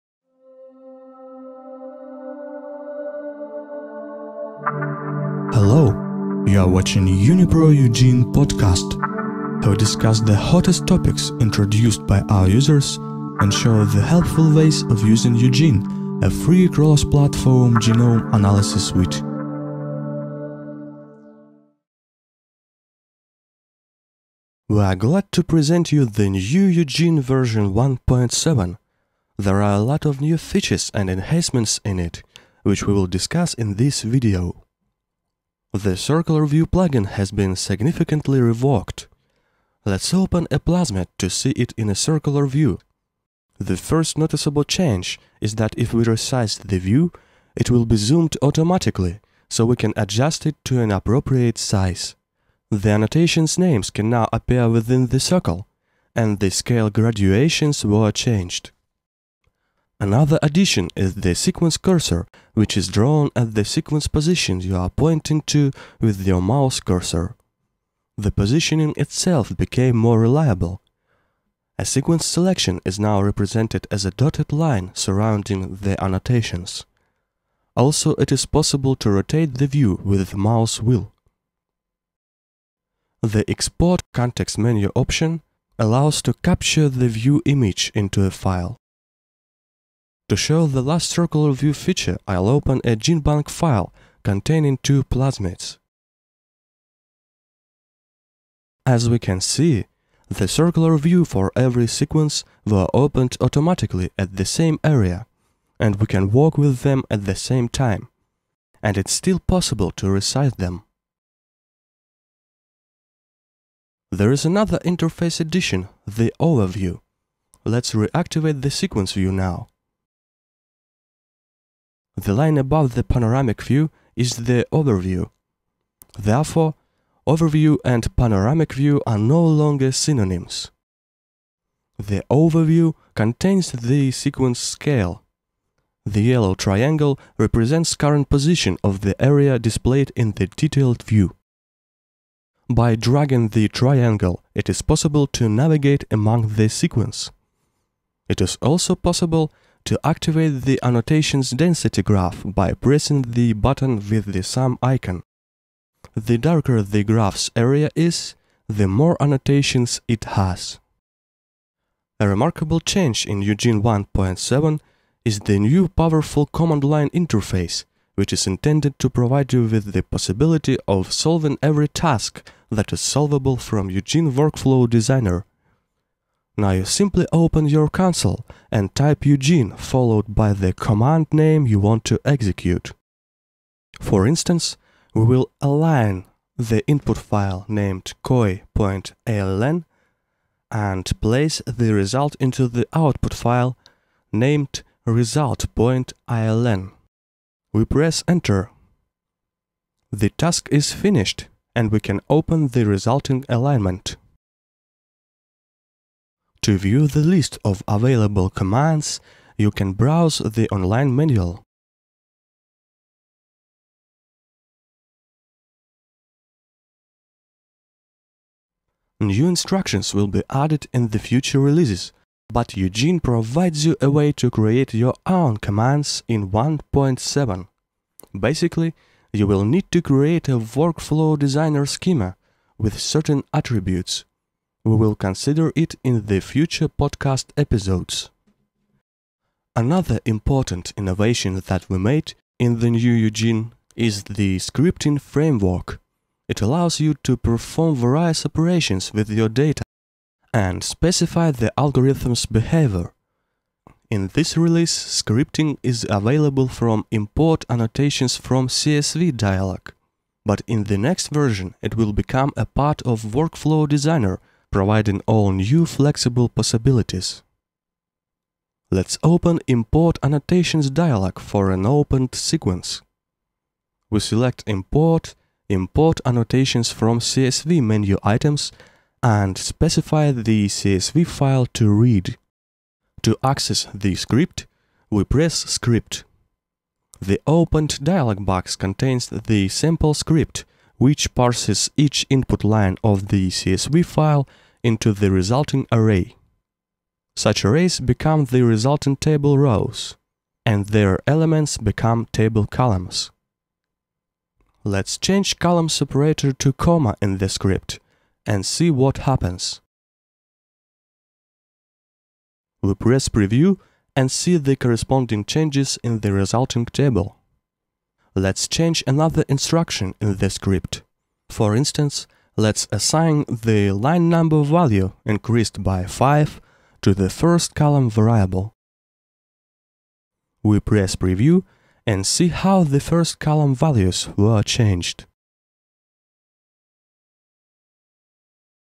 Unipro UGENE podcast #19: What's new in UGENE 1.7, part 1
The second „What's new in UGENE? video dedicated to the UGENE version 1.7 release.

The video goes in two parts. In the first part we consider significant Circular View enhancements, the added Overiew, new powerful command line interface and the added scripting framework.